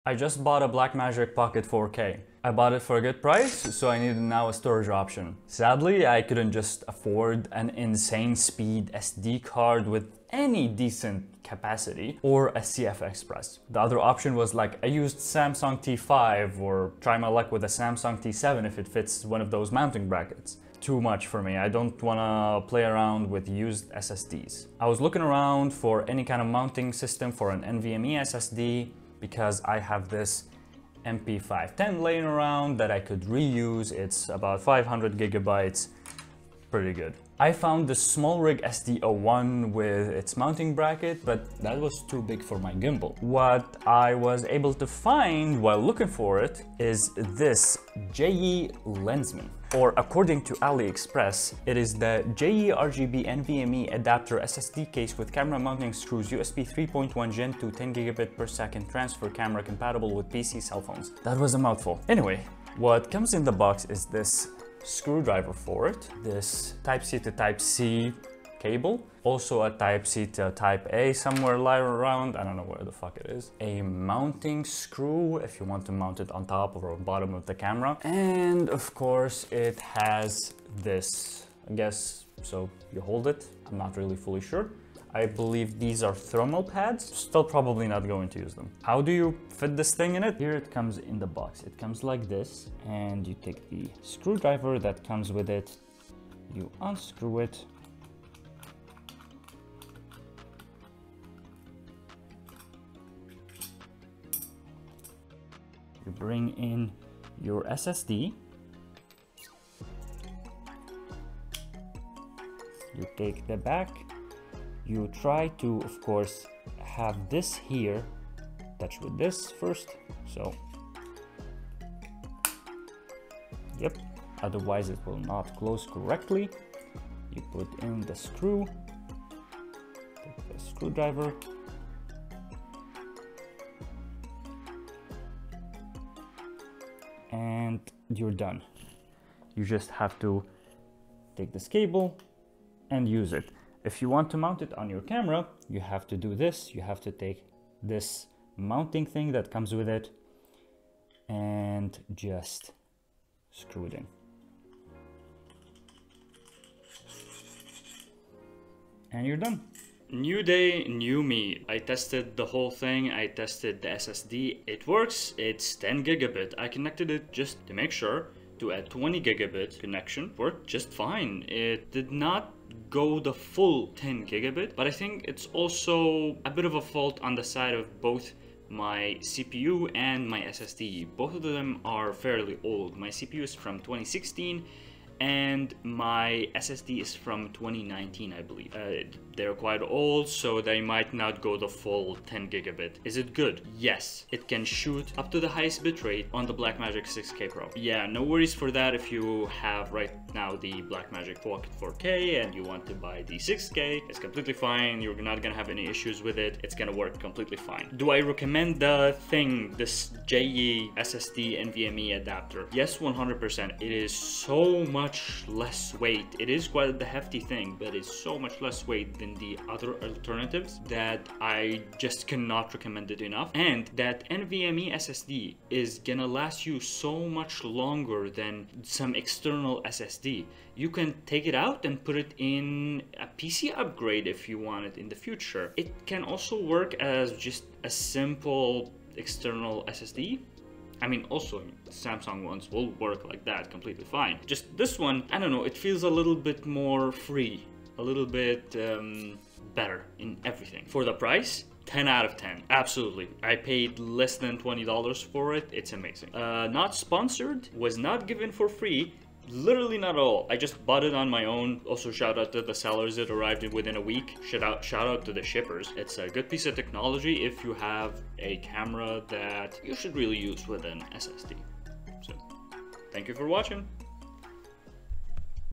Get THIS For Your Blackmagic 4K/6K - JEYI NVME SSD Camera Enclosure
JEYI M.2 NVMe SSD Enclosure with Camera Mounting Screw, USB 3.1 Gen2 10Gbps Transfer Camera Footage, Compatible with PC, phone is one title for this device.
This video is a quick review and was not paid nor gifted.